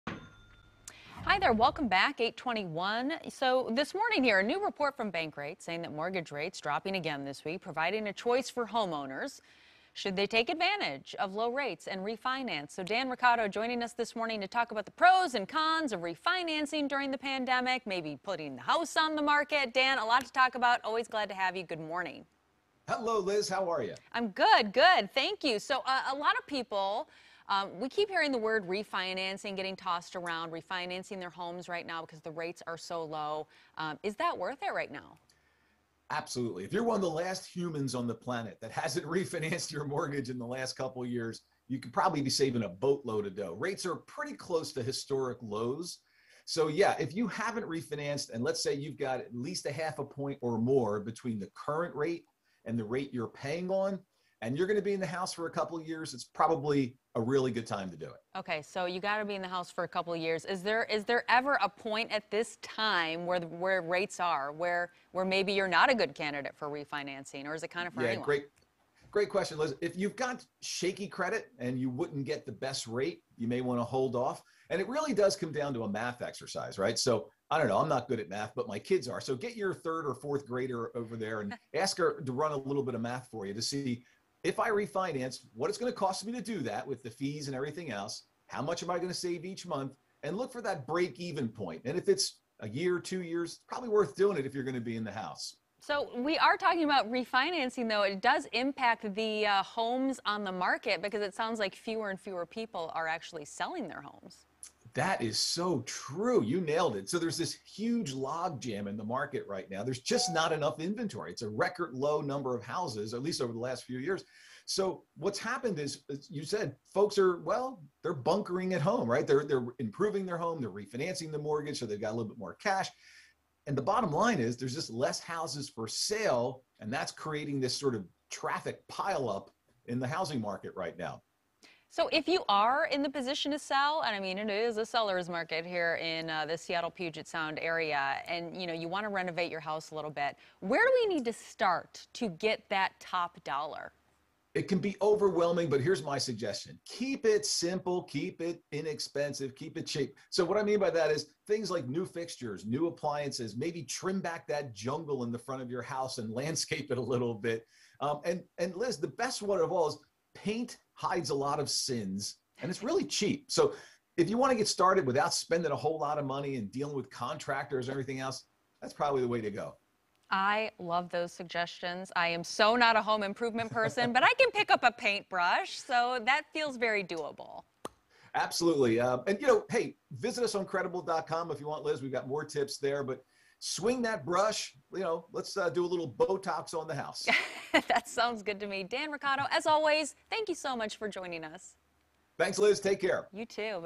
Refinancing your home during the pandemic
Dan Roccato with Credible joins Q13 News This Morning to talk refinancing amid historically low interest rates.